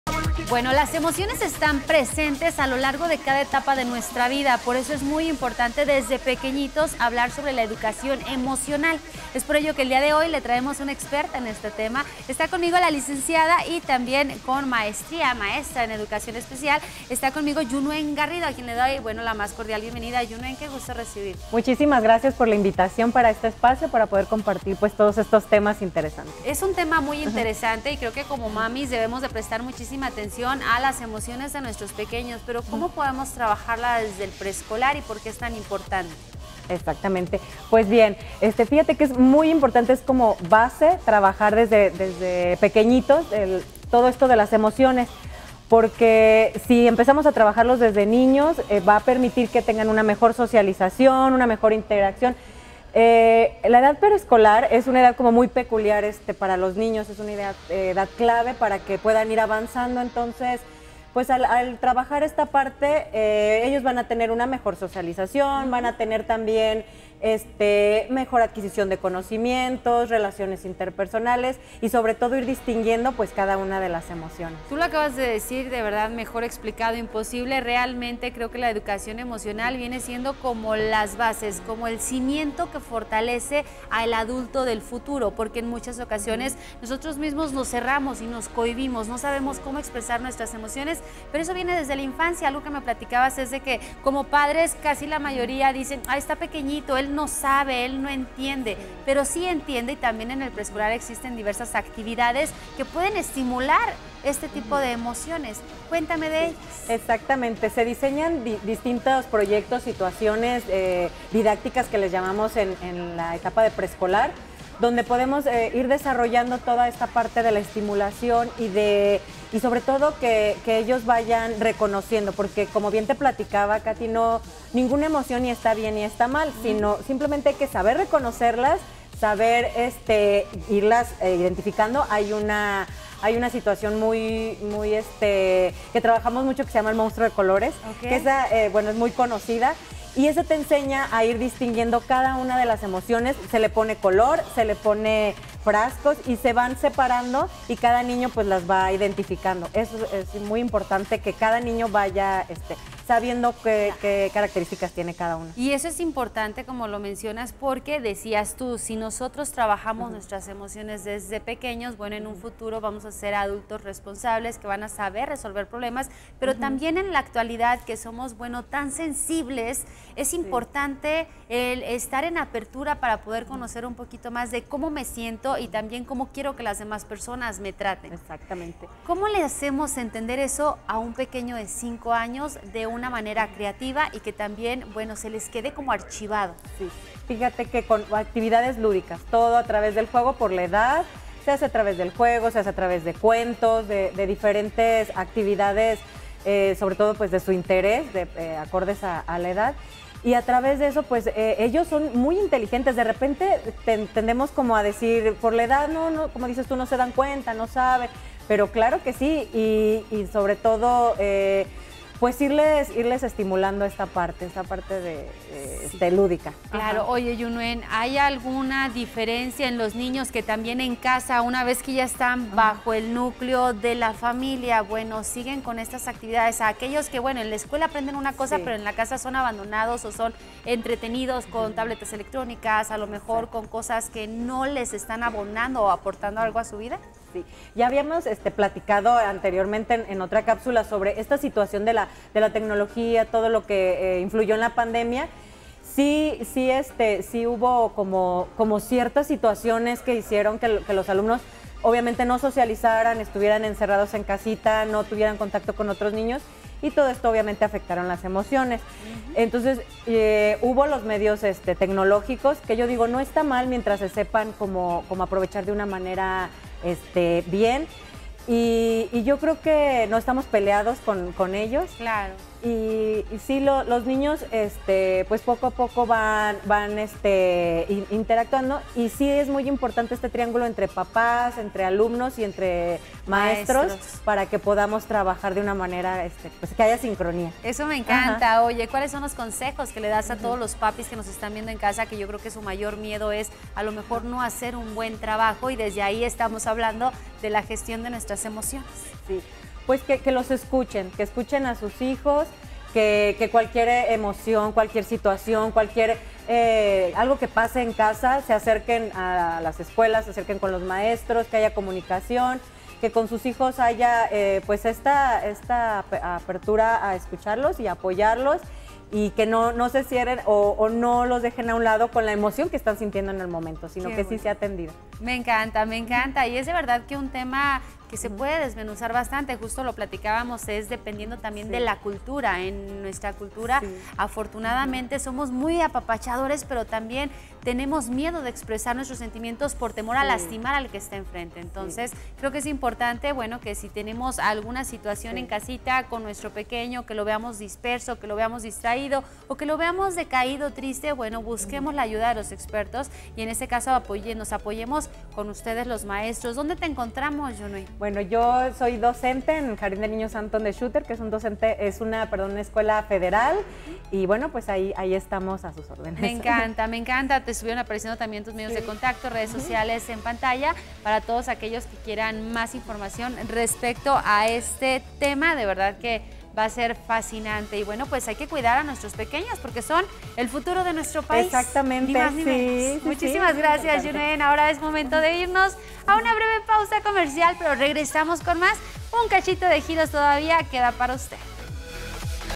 Educación Emocional Infantil | Giros Michoacán
Las emociones son parte de nuestro día a día, por ello tienes que conocer todo sobre la educación emocional desde la infancia 💛